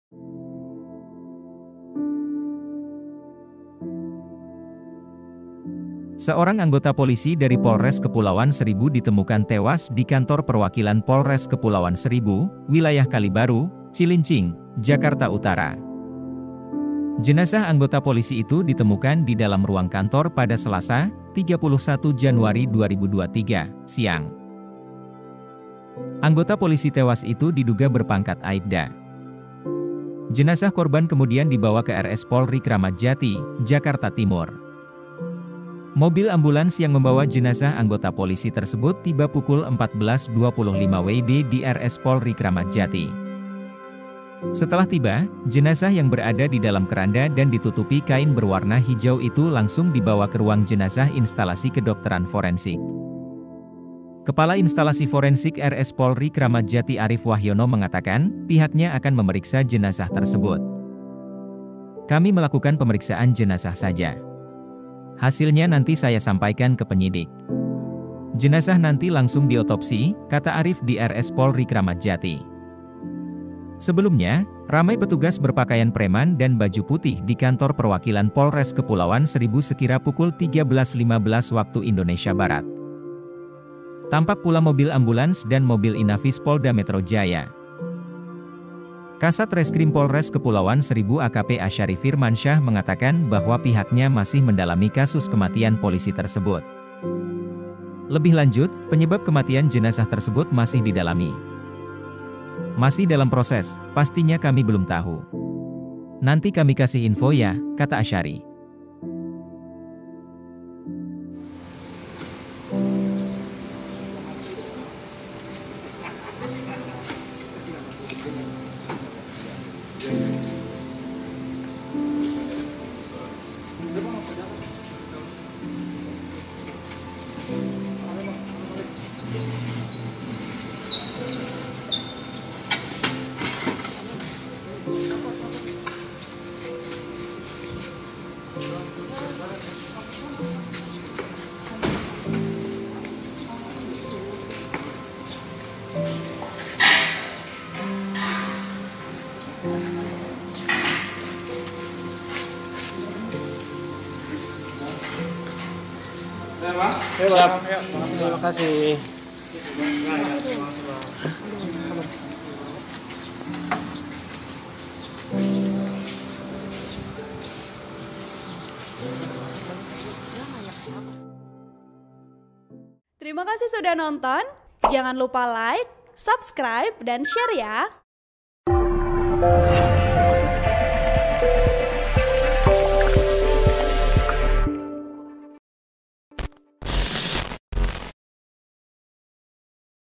Polisi Tewas di Kantor Perwakilan Polres Kepulauan Seribu, Penyebab Masih Dalami
POS-KUPANG.COM - Seorang anggota polisi dari Polres Kepulauan Seribu ditemukan tewas di Kantor Perwakilan Polres Kepulauan Seribu, wilayah Kalibaru, Cilincing, Jakarta Utara.

Jenazah anggota polisi itu ditemukan di dalam ruang kantor pada Selasa (31/1/2023) siang.  Anggota polisi tewas itu diduga berpangkat Aipda. (POS-KUPANG.COM, Ryan Nong)

Artikel ini telah tayang di Pos-Kupang.com dengan judul Polisi Tewas di Kantor Perwakilan Polres Kepulauan Seribu, Penyebab Masih Dalami

Source  : Facebook Tribun Jakarta